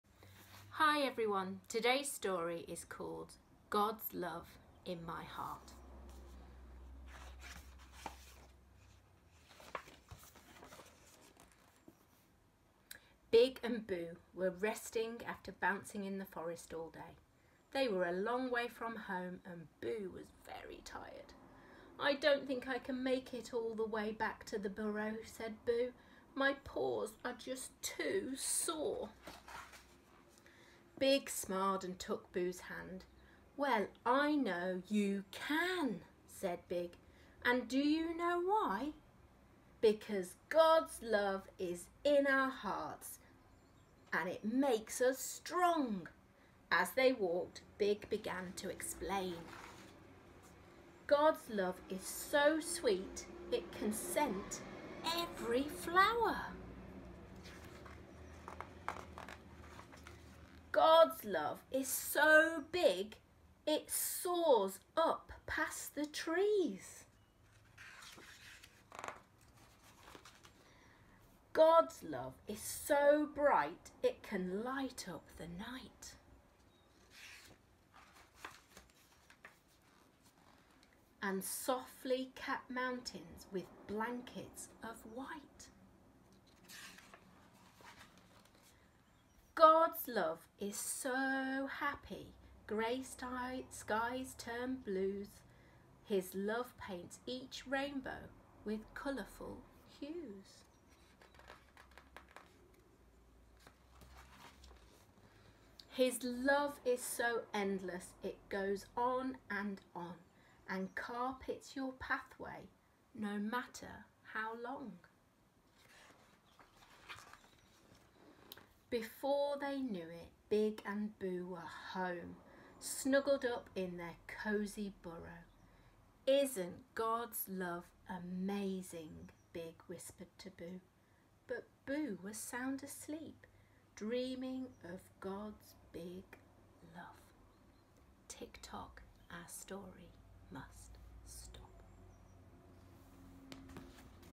God's big love story
A story about Big and Boo and God's great love for them.